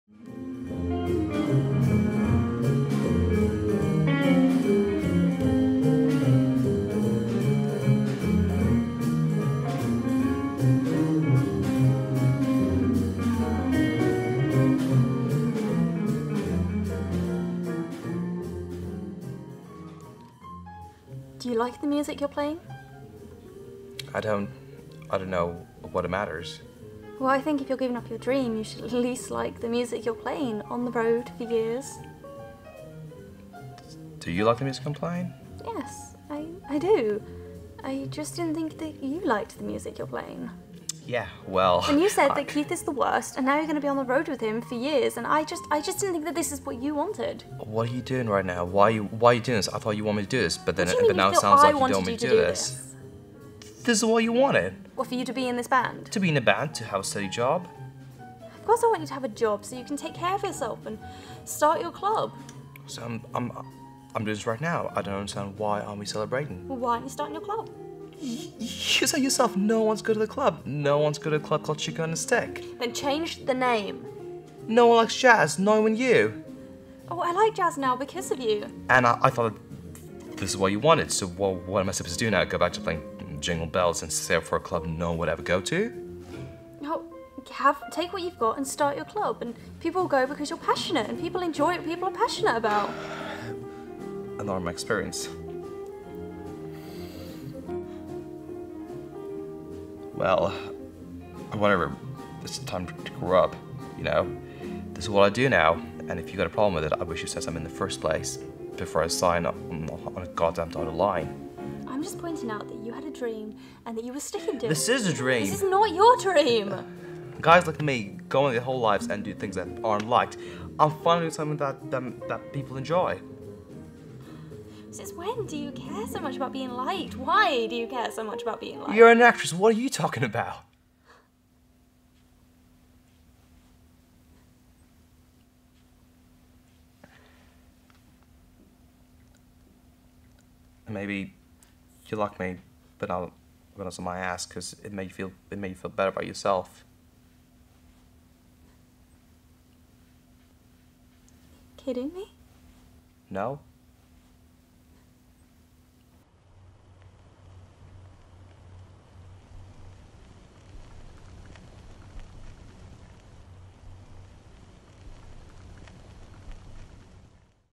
"La La Land" Argument Scene Recreation (2019)
Another short film project, this time to recreate a dialogue sequence from a film, in terms of lighting the scene and characters and camera angles etc.

Completed: February 6th 2019